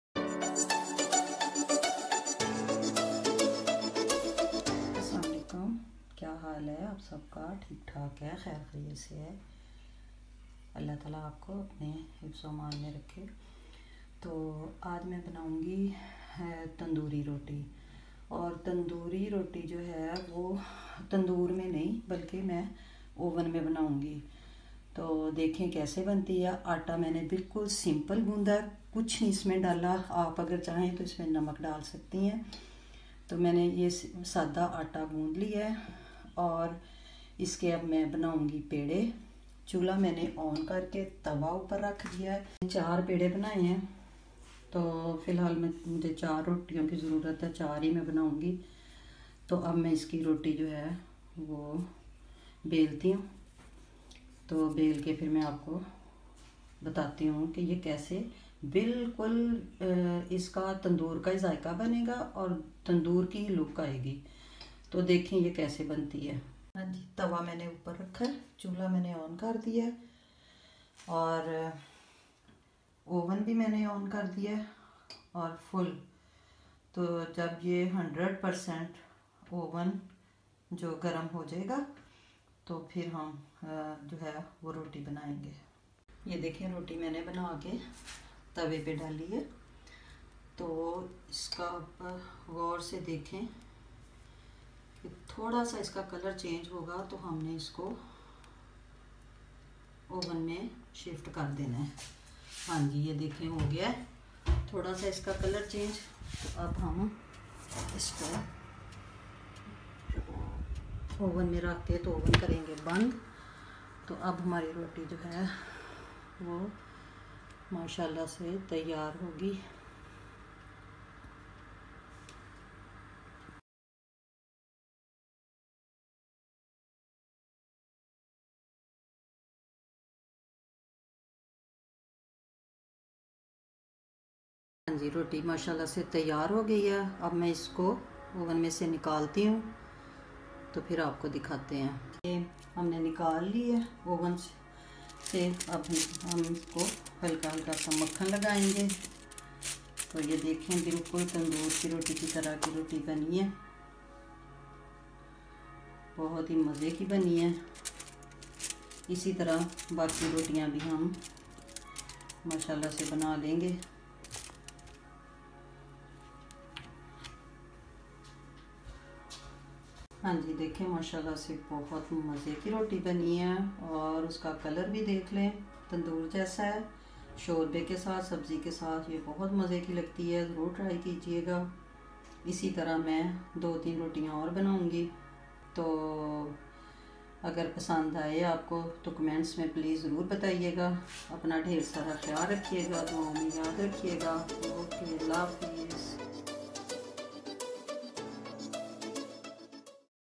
TANDOORI ROTI without tandoor
Here I'm  sharing a easy way to make delicious tandoori roti in oven.